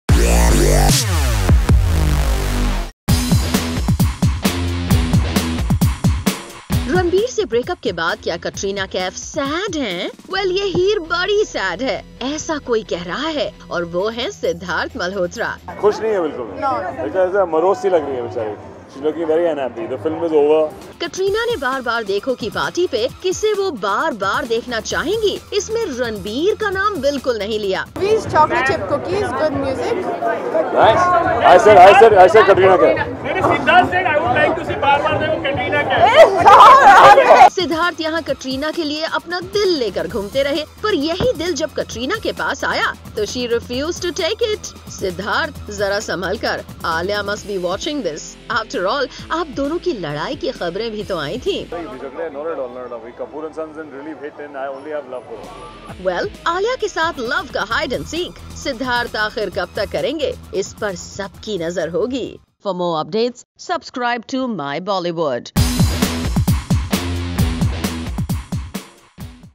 ‎Sidharth Malhotra‬ Adresses Why ‪‎Katrina Kaif‬ Has Been Really low! | Bookmybollywood
Siddharth Katrina come together for Baar Baar Dekho